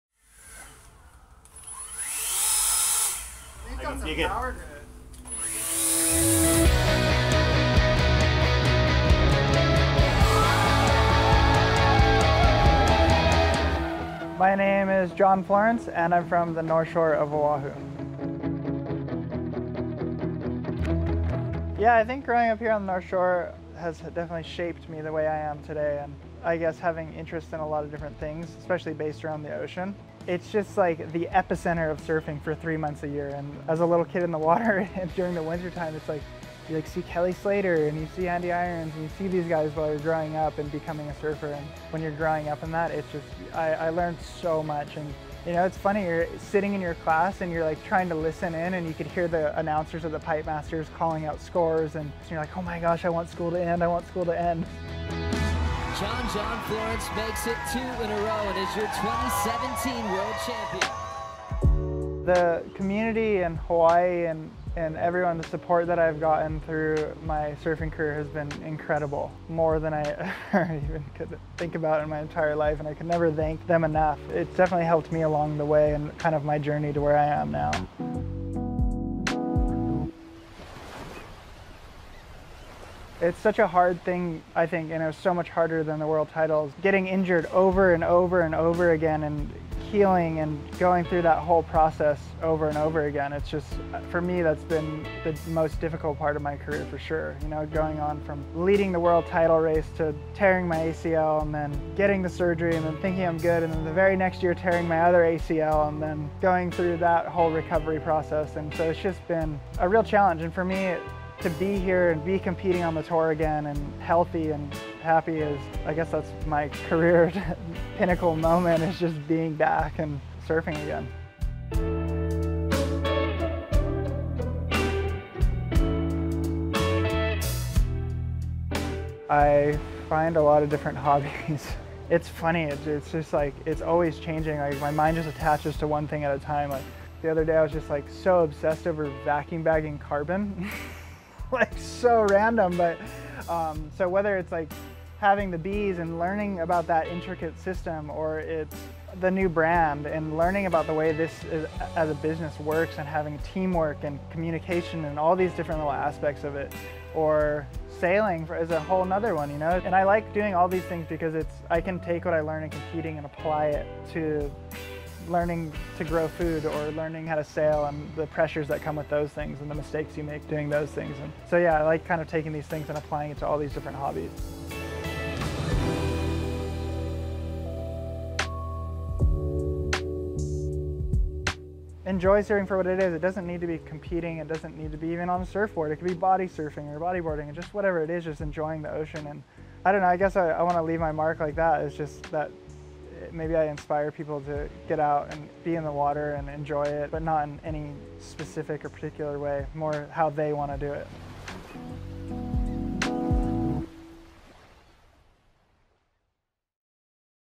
Meet Championship Tour Surfer John John Florence
Meet Championship Tour surfer John John Florence and get a deeper look at what drives him in and out of the water.